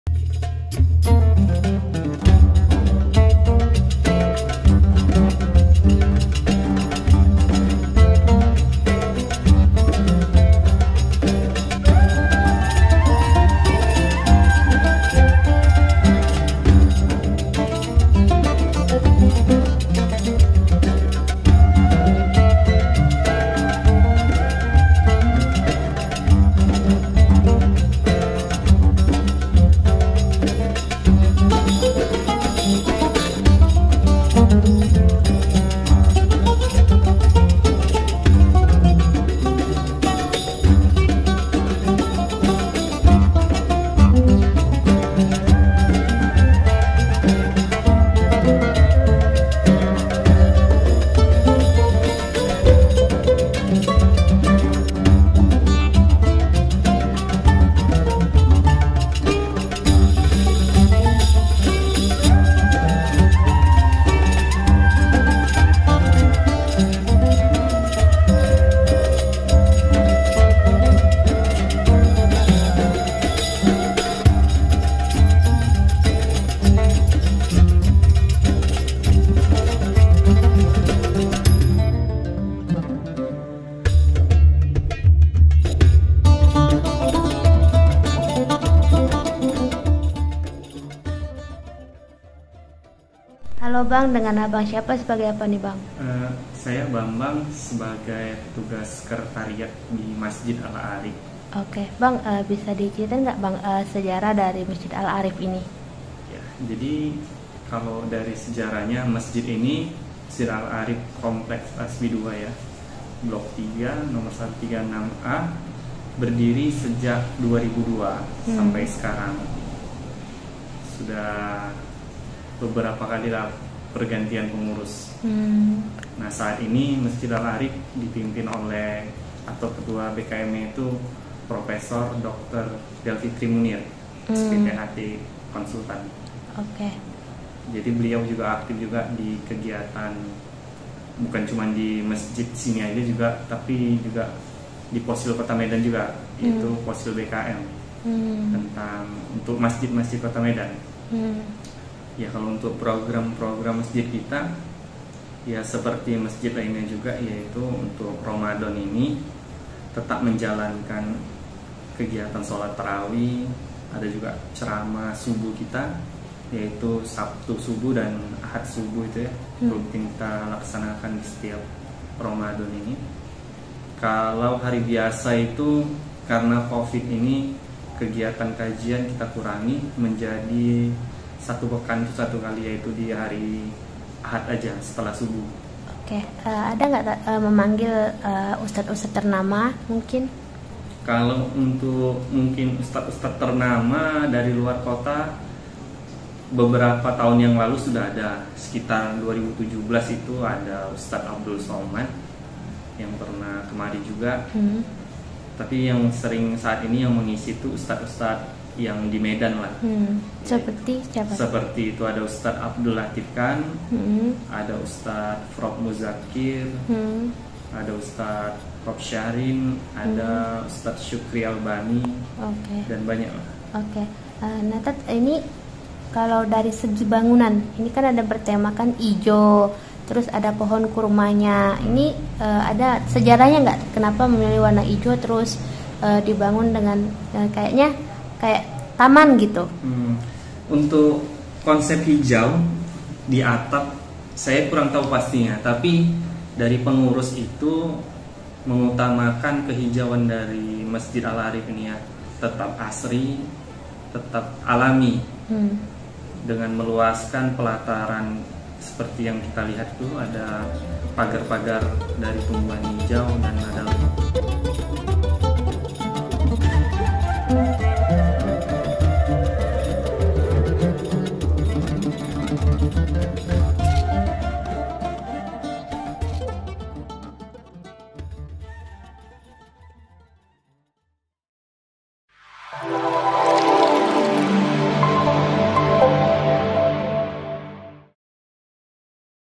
Keindahan Masjid Al'Arif Dengan Konsep Asri Dan Sejuk, Miliki Dua Batang Pohon Kurma di Halamanya
TRIBUN-MEDAN.com, MEDAN - Saat ini banyak bangunan masjid yang menampilkan keindahan dengan bangunan yang elegan dan menarik.

Namun ada juga beberapa masjid yang masih menampilkan kesederhanaan bangunan tetapi tetap menghadirkan suasana nyaman saat memasukinya.

Misalnya saja Masjid Al-Arif di Komplek tasbih 2 blok 3 no 136 A Jalan Ring Road, Asam Kumbang Kecamatan Medan Selayang Kota Medan.

Dari amatan Tribun Medan, masjid ini bernuansa suasana sejuk nan asri terlihat dari kubah masjid berwarna hijau, tiang-tiang ber keramik hijau berpadu padankan dengan warna putih.

Begitupun bagian dalam masjid hanya bekonsep bangunan sederhana berwarna putih.

Namun uniknya di halaman masjid ini dipenuhi dengan berbagai macam tanaman sehingga membuat masjid terlihat asri.